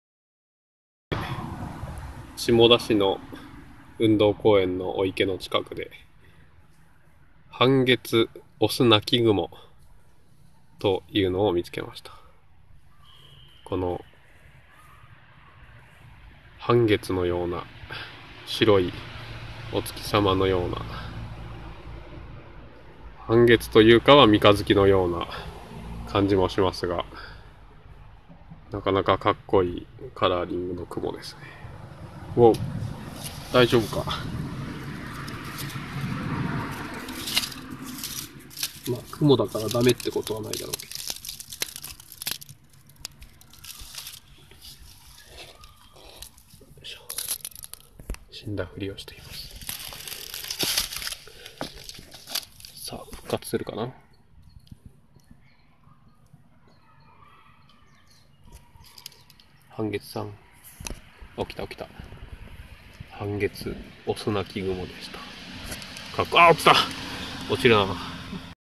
ハンゲツオスナキグモを見つけたよ！
白い部分は、半月よりも三ヶ月くらいの薄さですね